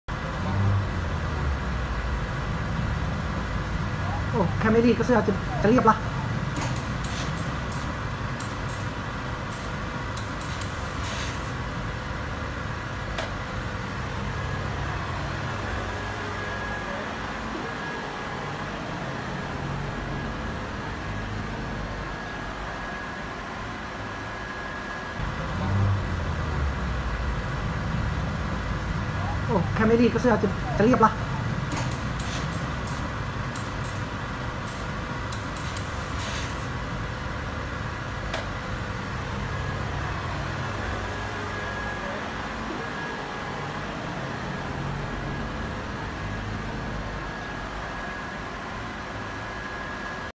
Vacuum Ironing Table with Boiler Steam Iron B23
Vacuum Ironing Table B20
🚩รีดเสื้อผ้าได้ทุกชนิด
🚩รีดผ้าโหลมือ2
🚩รีดผ้าปูที่นอน
🚩รีดผ้าม่าน
🚩 แข็งแรง ทนทาน ใช้งานต่อเนื่องได้ 12 ชั่วโมง
รับประกันอะไหล่/บริการ 1 ปีฟรี
คุณแหวน
🚩 แข็งแรง ทนทาน ใช้งานต่อเนื่องได้ 24 ชั่วโมง
คุณแหวน